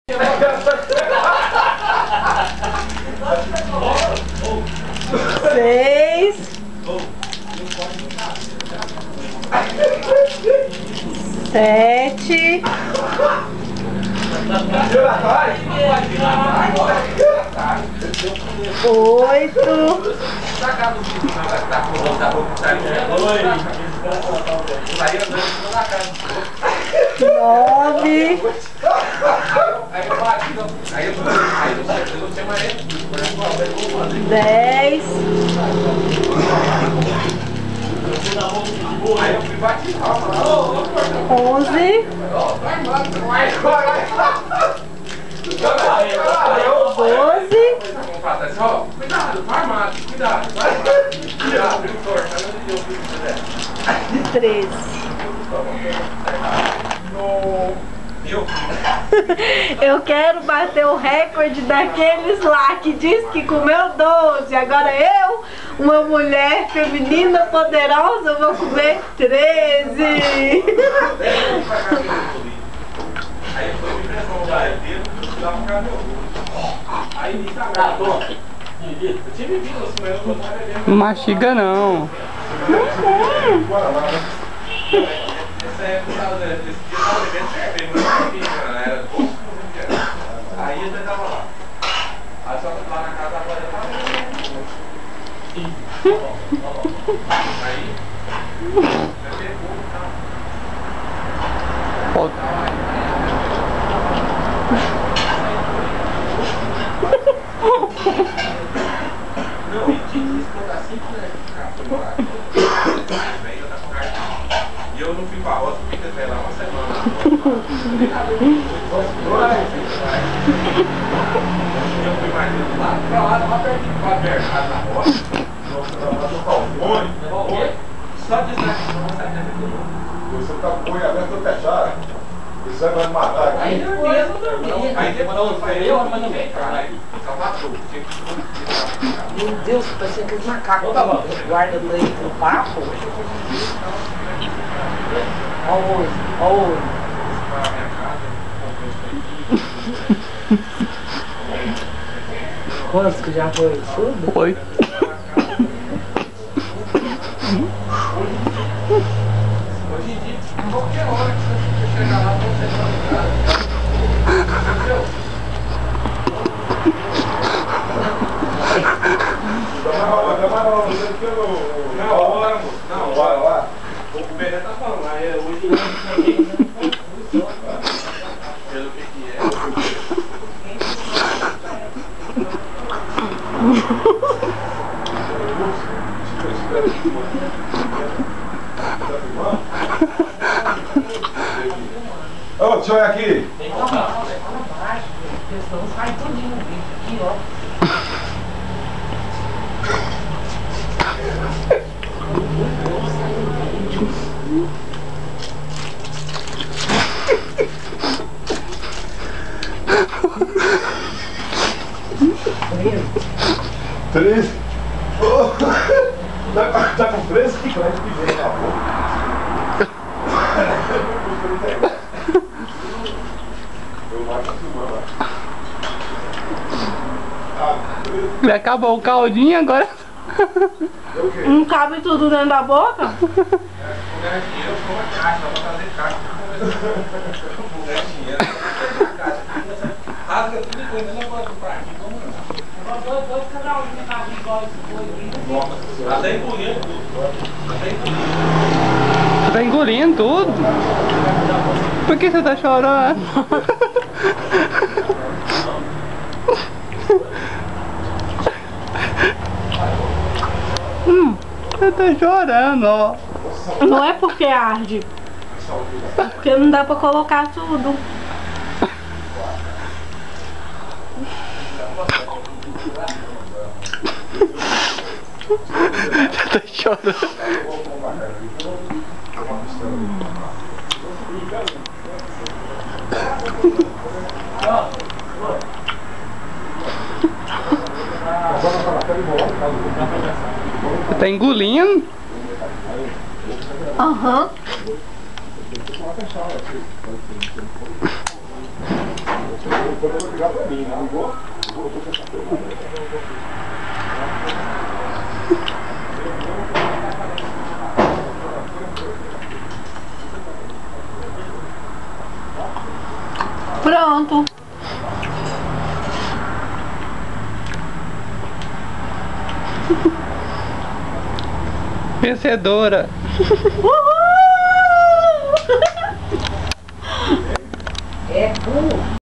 Mychelle Mendes 13 Kriptonita EXTREME (minha namorada ela e D+)
Ela bateu o atual Record que era de 12 kriptonita, ela comeu literalmente 13 kriptonita sabor extreme
Ela e dimais
amor voce e otimaaaaaa dalhe choro hauihuaihaiuhiuahuiha

vamos ver da proxima vc chupando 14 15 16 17...

huiauhiahiauhiha